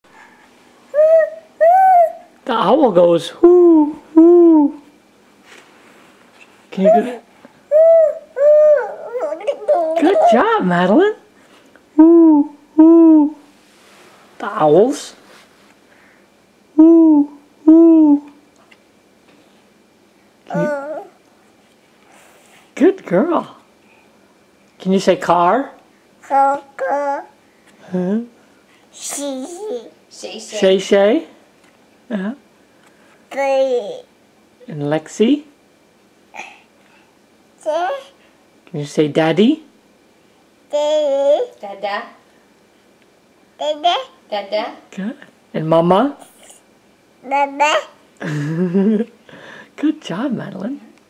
Madeline's First Words
15 mo old Madeline hooting like an owl and saying Car, Shae Shae (the dog), Daddy, and Mommy.